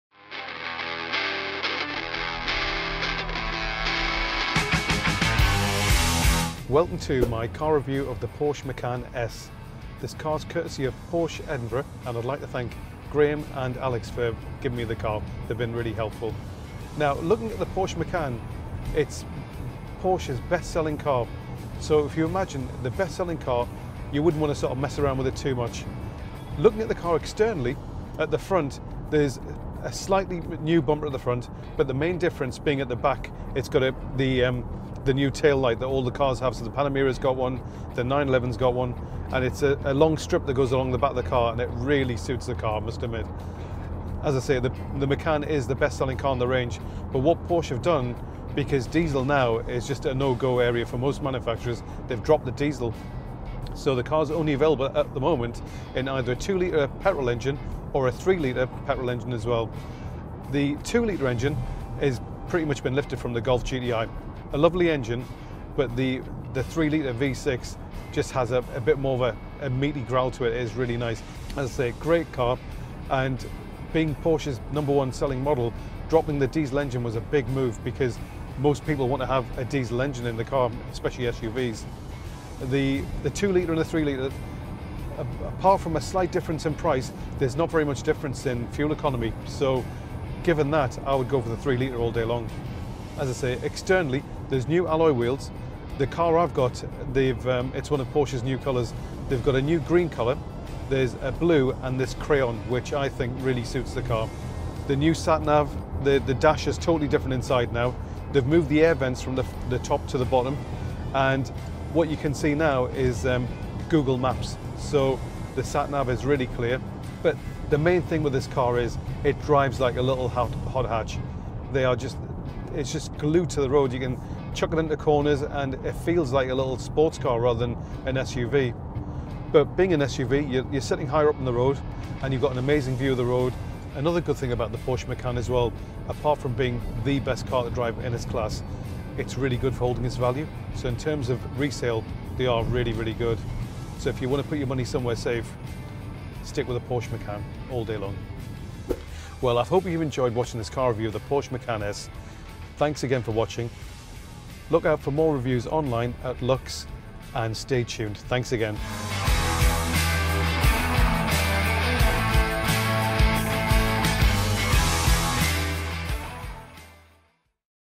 At The Wheel: Porsche Macan
Bob Arora gets behind the wheel of a Porsche Macan for Luxe magazine